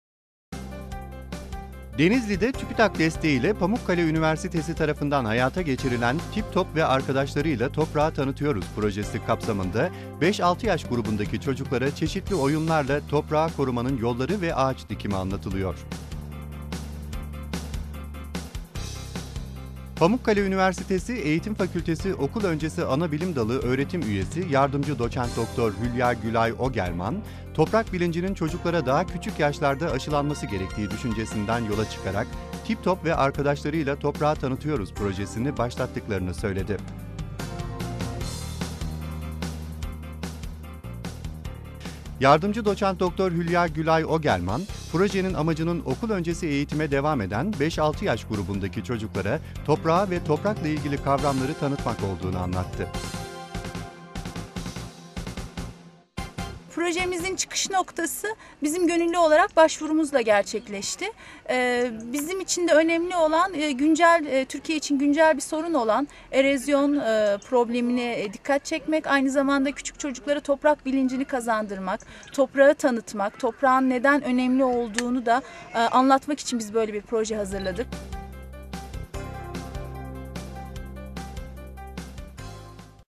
EROZYONU OYUNLA ÖĞRENİYORLAR 11.11.2011.wmv
Denizli'de TÜBİTAK desteğiyle Pamukkale Üniversitesi tarafından hayata geçirilen ''Tiptop ve Arkadaşları ile Toprağı Tanıtıyoruz Projesi'' kapsamında 5-6 yaş grubundaki çocuklara çeşitli oyunlarla toprağı korumanın yolları ve ağaç dikimi anlatılıyor.
Pamukkale Üniversitesi (PAÜ) Eğitim Fakültesi Okul Öncesi Anabilimdalı Öğretim Üyesi Yrd. Doç. Dr. Hülya Gülay Ogelman, toprak bilincinin çocuklara daha küçük yaşlarda aşılanması gerektiği düşüncesinden yola çıkarak ''Tiptop ve Arkadaşları ile Toprağı Tanıtıyoruz Projesi''ni başlattıklarını söyledi.
Yrd.Doç.Dr. Hülya Gülay Ogelman, projenin amacının okul öncesi eğitime devam eden 5-6 yaş grubundaki çocuklara toprağı ve toprakla ilgili kavramları tanıtmak olduğunu anlattı.